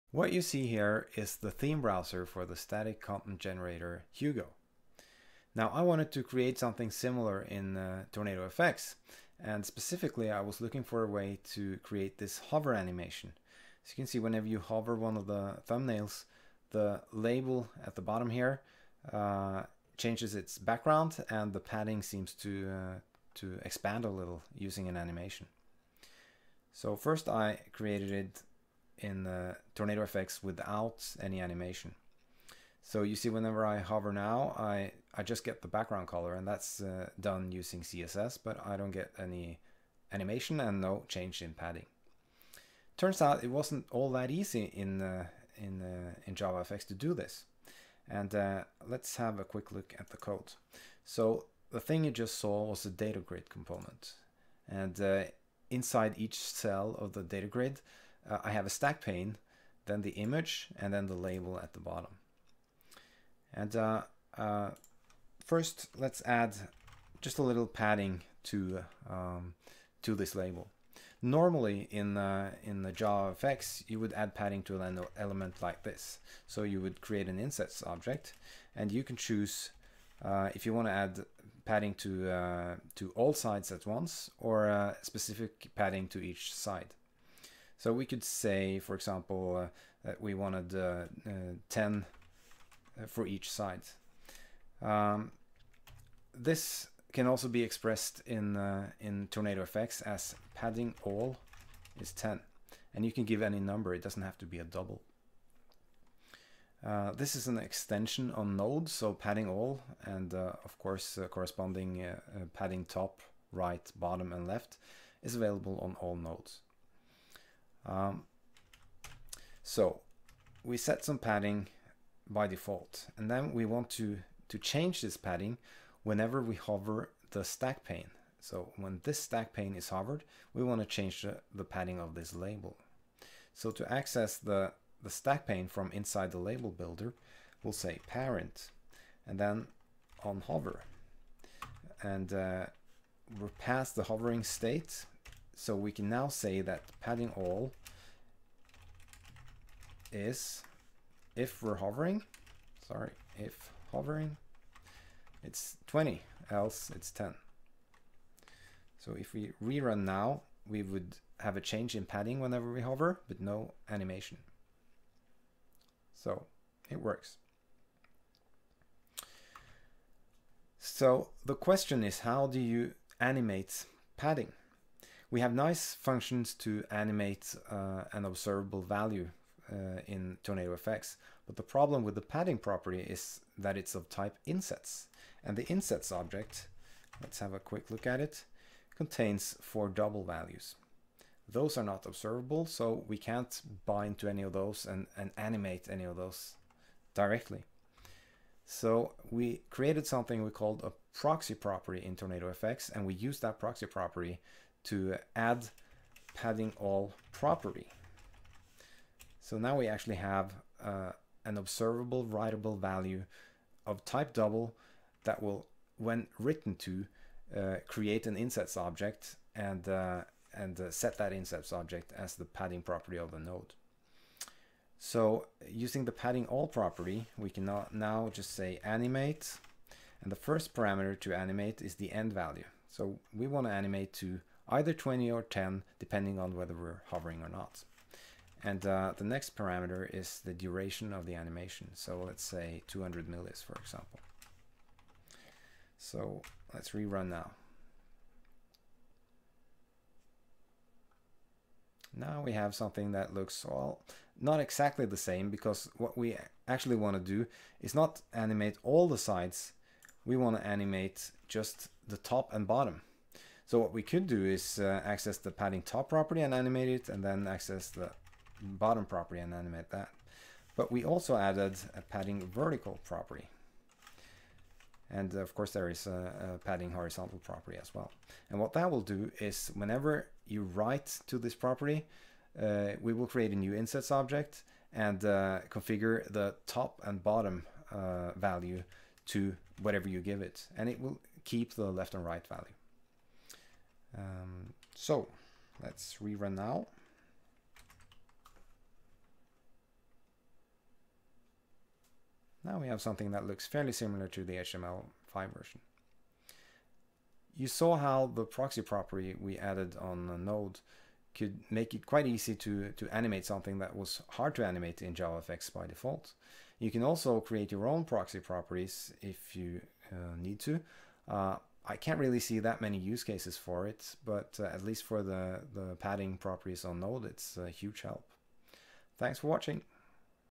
Animate padding in TornadoFX using proxy properties
Animating padding in JavaFX is not trivial, because padding is expressed as an Insets object which doesn't contain observable values. We added some extension functions to TornadoFX to make it easier to animate padding, and the technique we used might have even more use cases :)